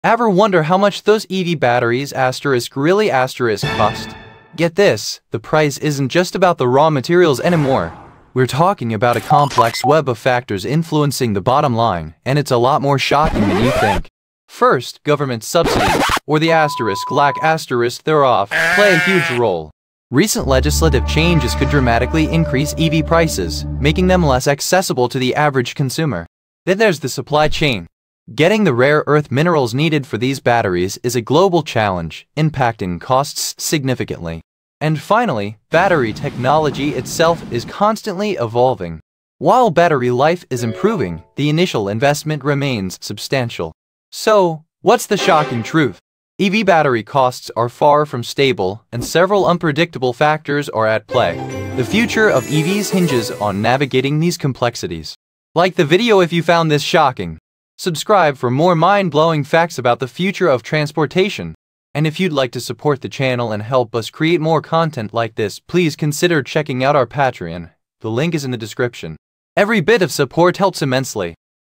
Shocking Truth About EV Battery Costs (2024 Update)
🤯  Think EV batteries are just about the materials? Think again!  This short reveals the SHOCKING hidden costs – from government subsidies to supply chain nightmares.  Uncover the REAL price of electric vehicles and prepare to be amazed!